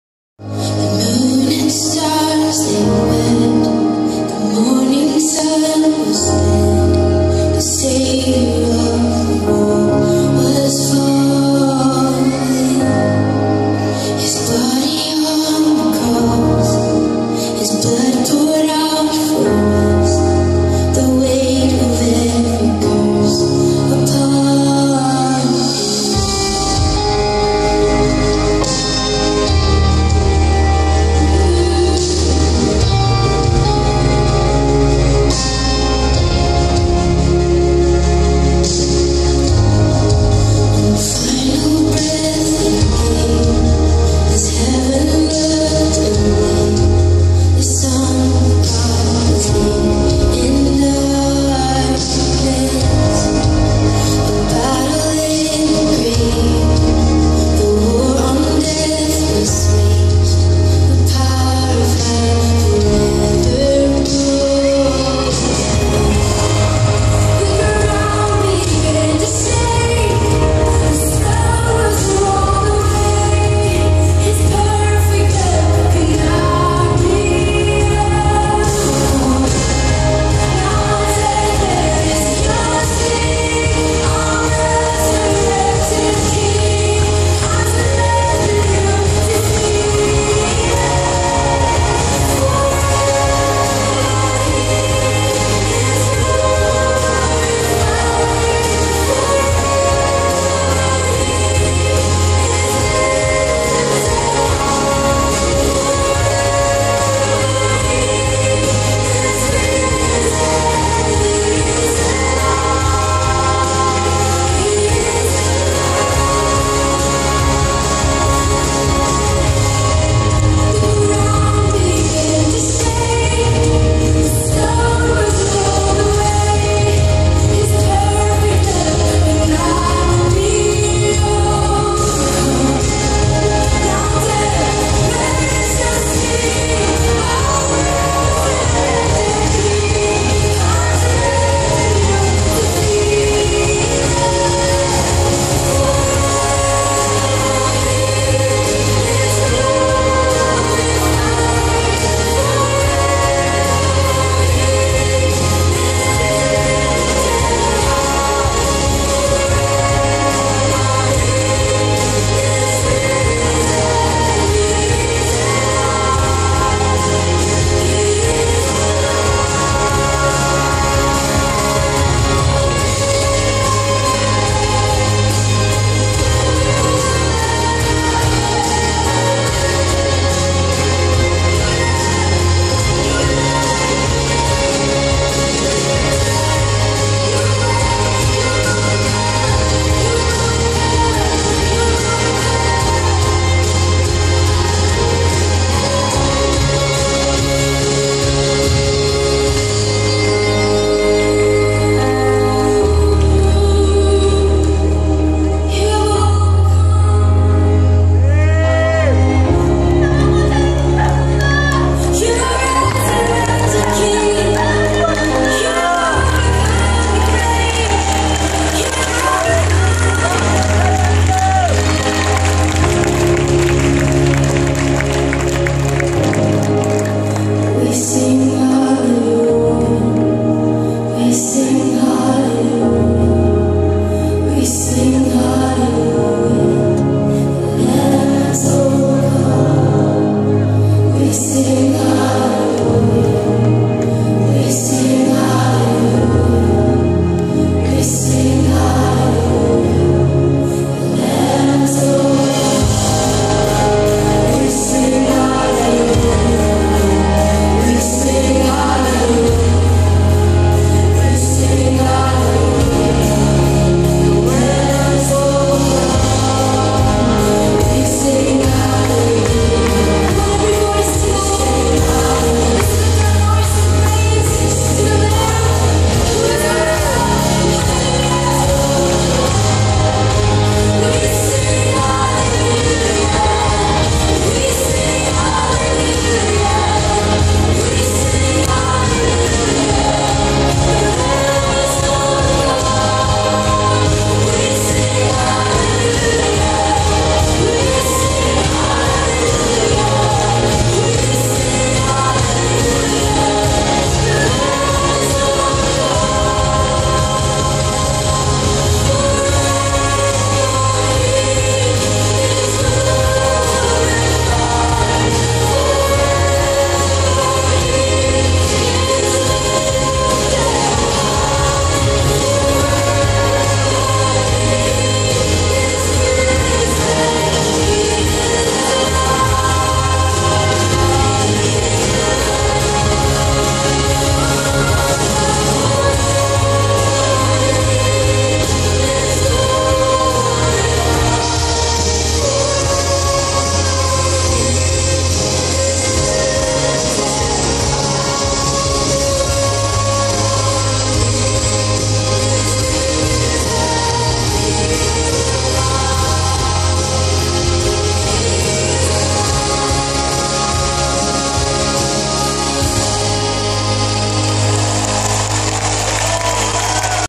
FLministry at celebration for the nations 2015-1
Father's Love dance at Celebration for the nations 2015 in Korea